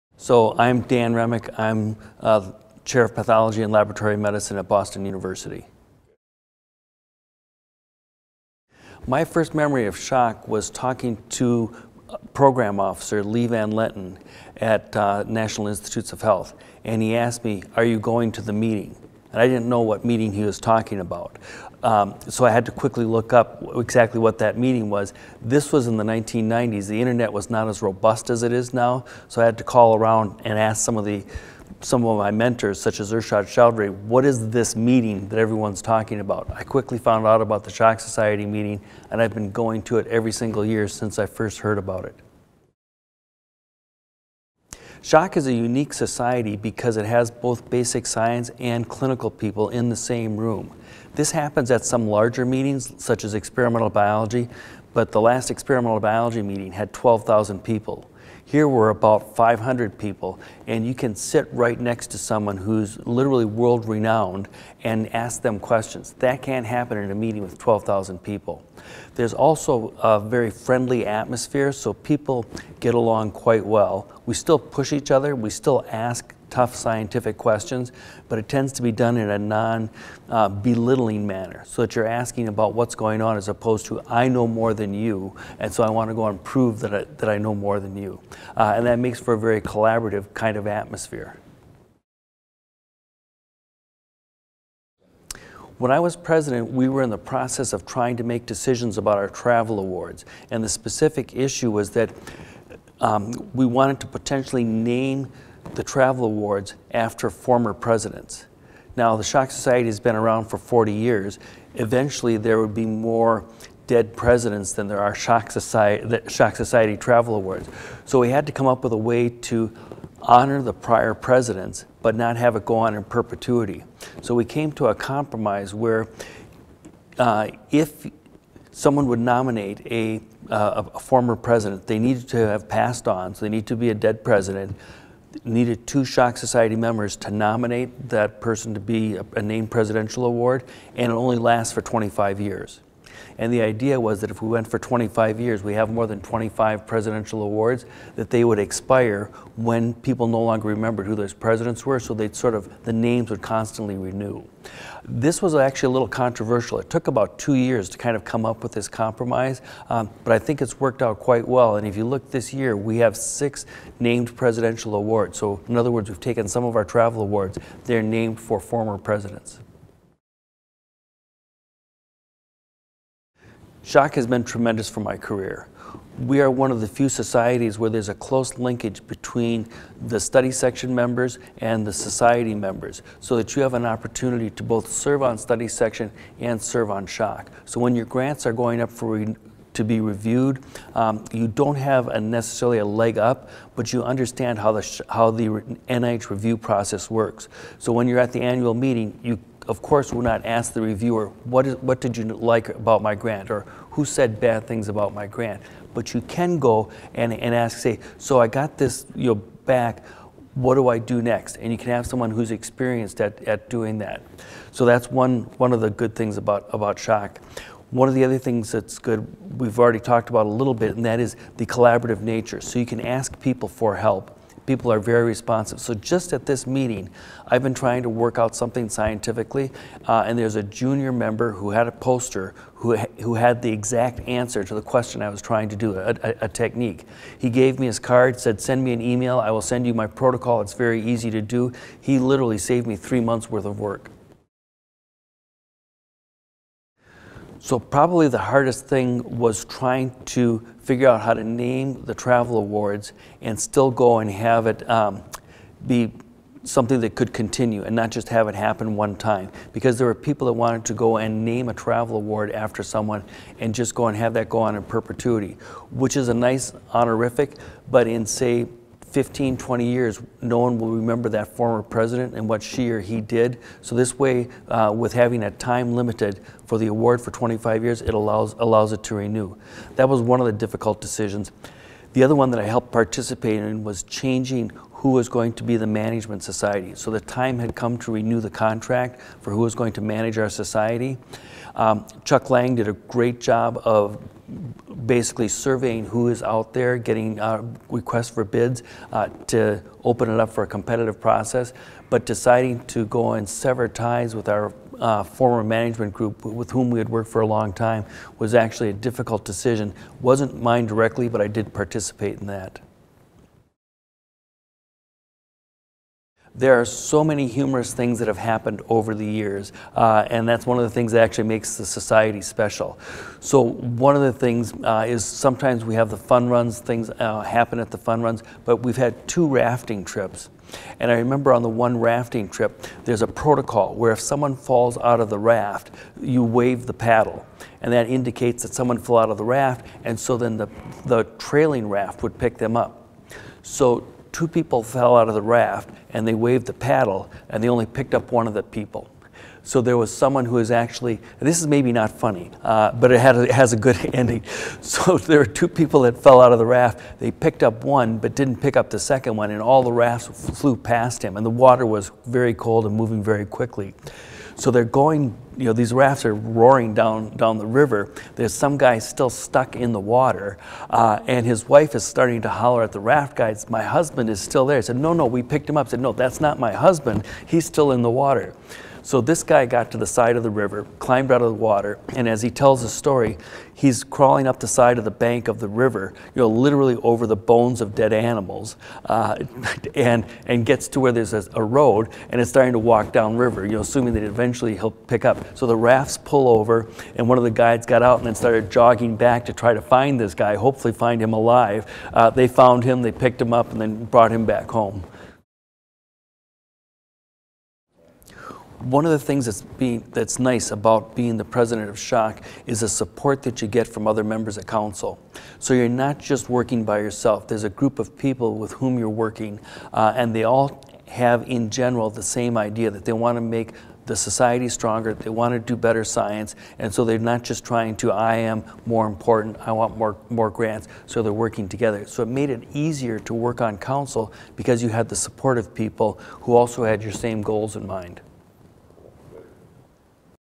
President's Video: Dan Remick, M.D.
Watch as Dan Remick, M.D. describes his time as Shock Society president for the 2009-10 term!